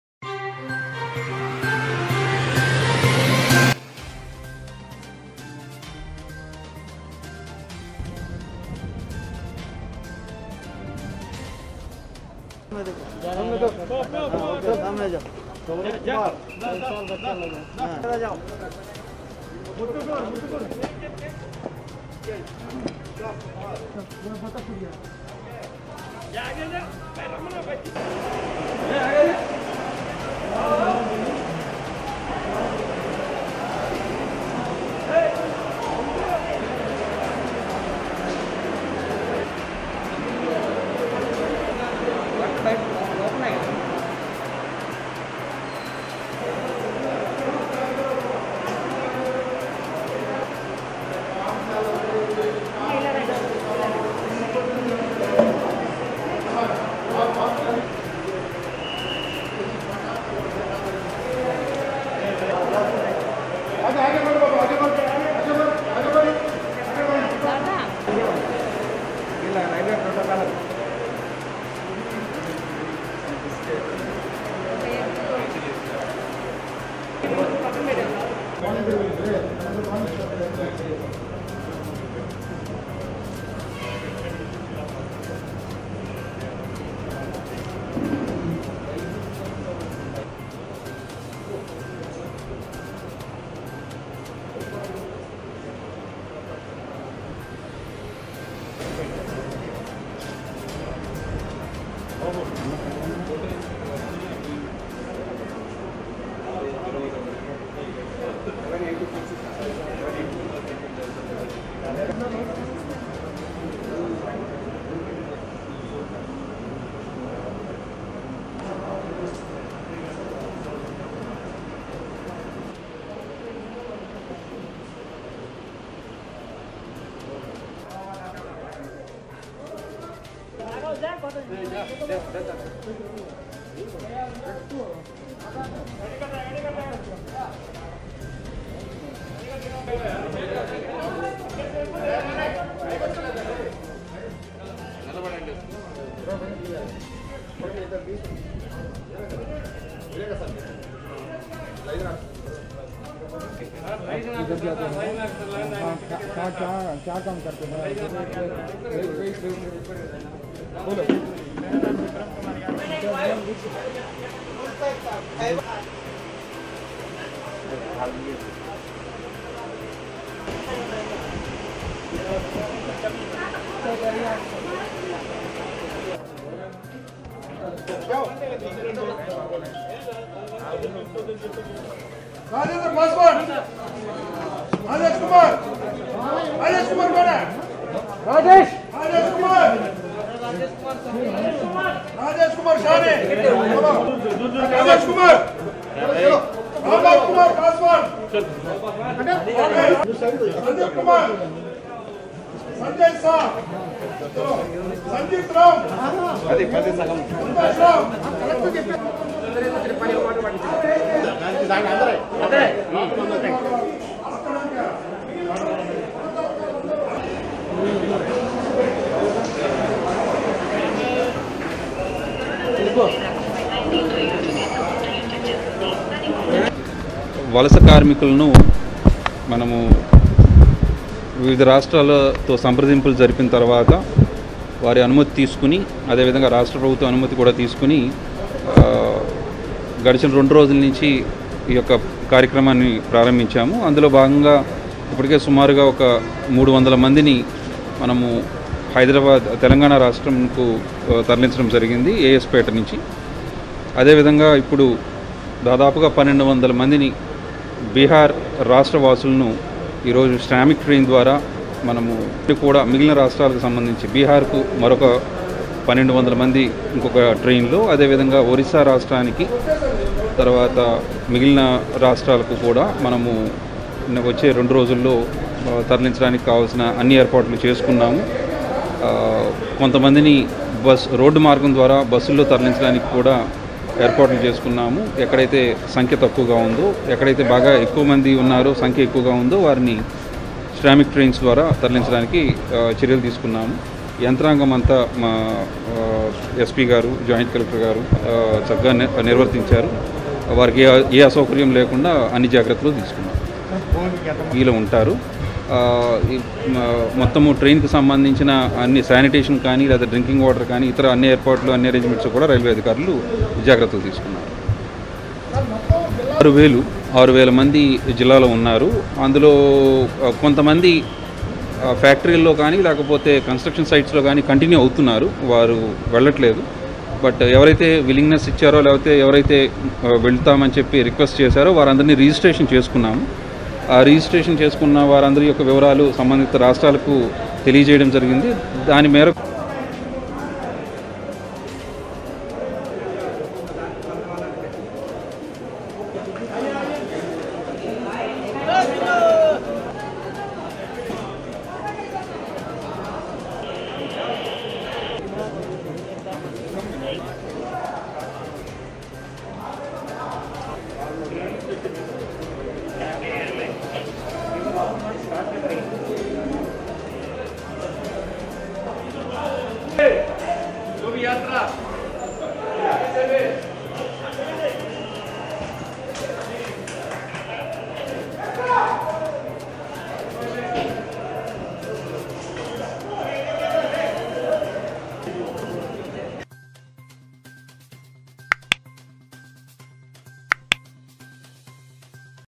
Lockdown in Nellore is a program to send migrant workers back to their hometowns
నెల్లూరులో లాక్ డౌన్ నేపద్యంలో వలస కార్మికులను వారి స్వస్థలాలకు పంపే కార్యక్రమం